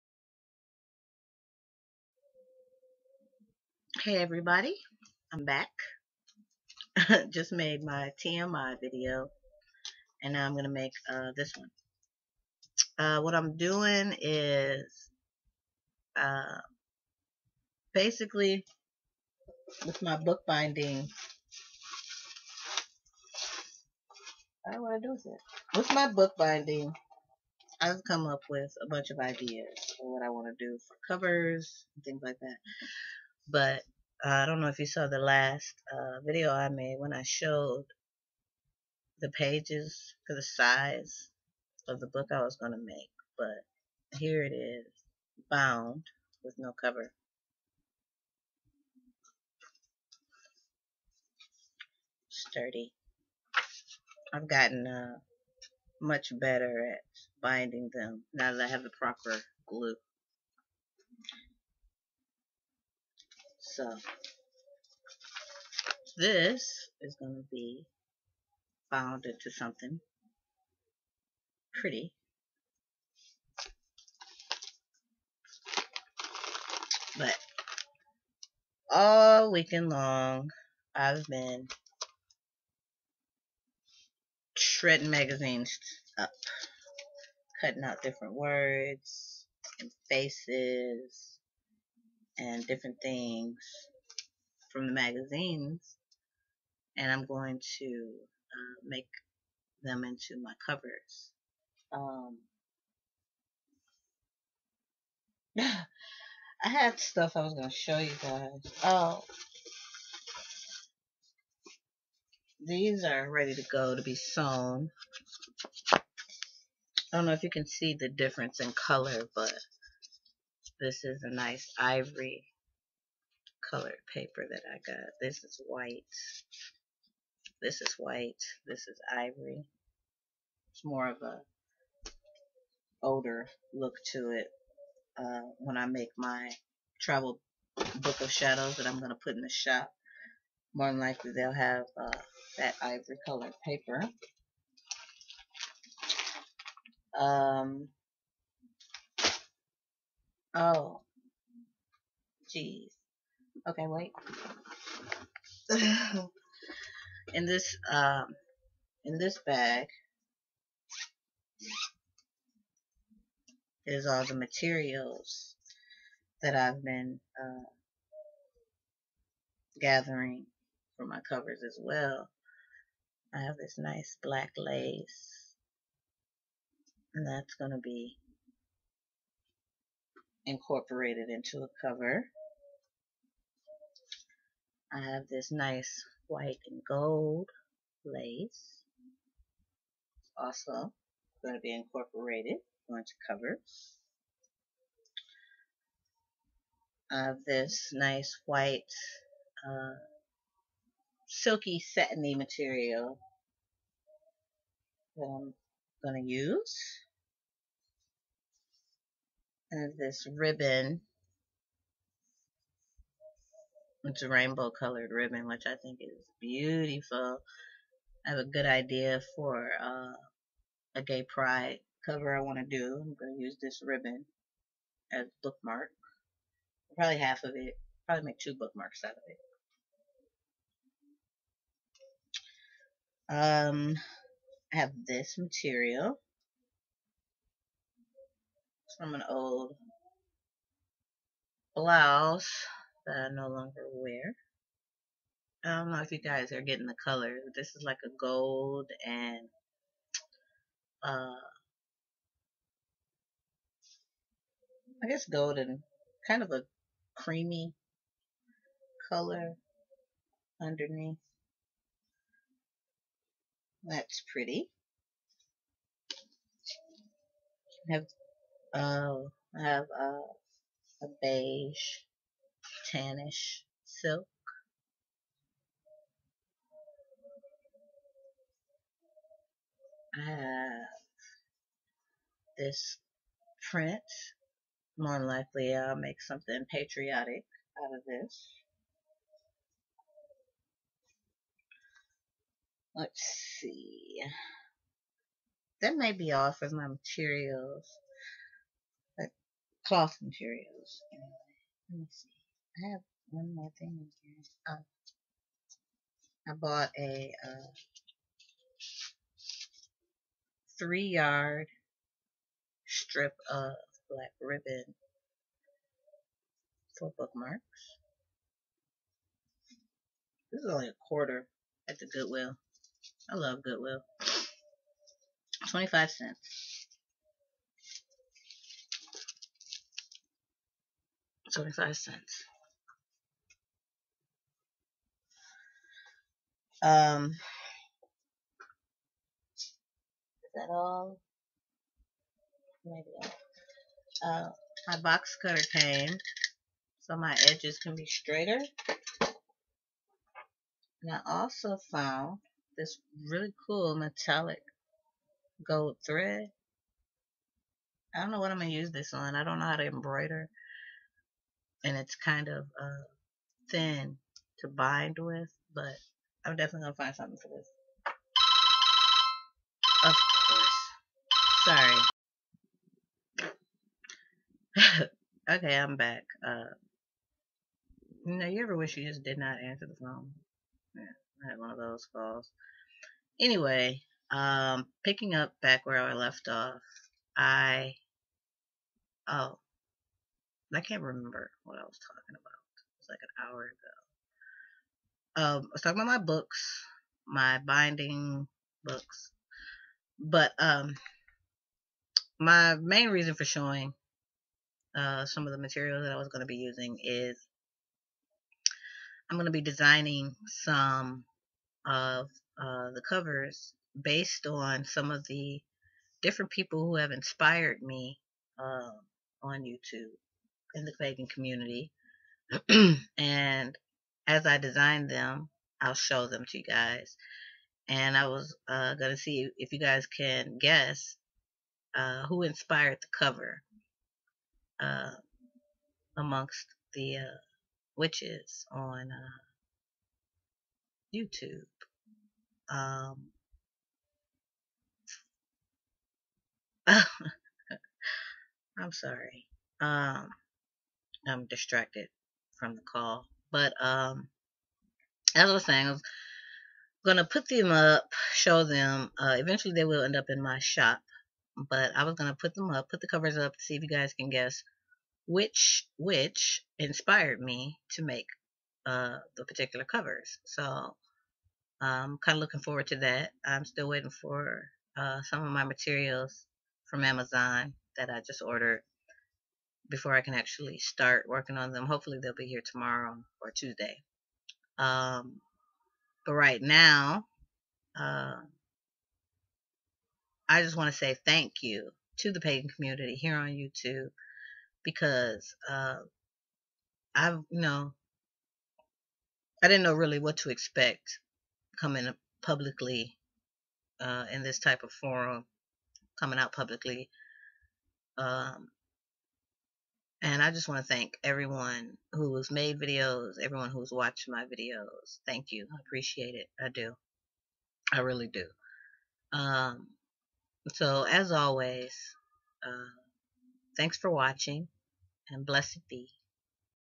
Ideas and Thanks
I have really been working hard on making products for my shop. I want to do some inspired covers eventually  and see if you guys can guess who inspired the cover. The books I make will be perfect for a small or travel sized BOS or simply a journal.